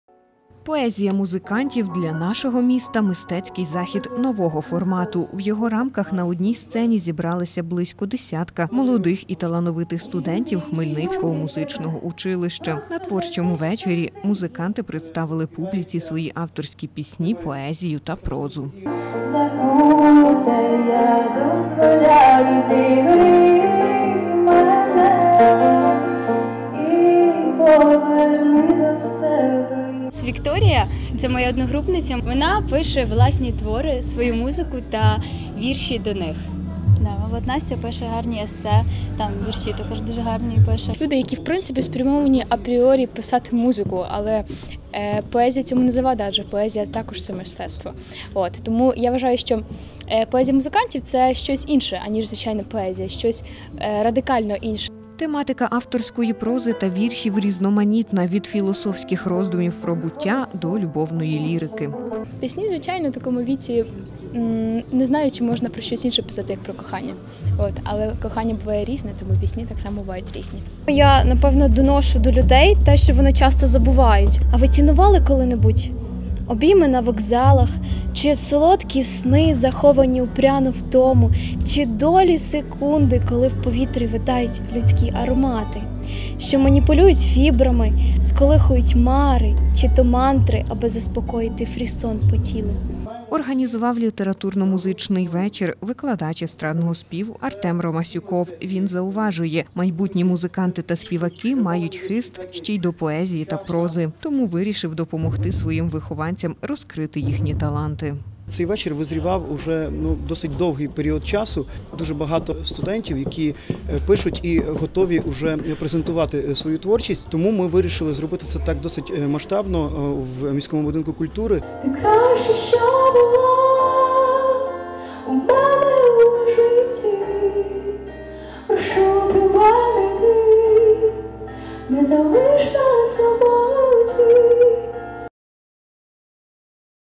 ХОДТРК "Поділля-центр" - Молоді таланти
Авторські пісні, поезія та проза. Вихованці музичного училища імені Владислава Заремби провели творчий вечір у Хмельницькому міському будинку культури.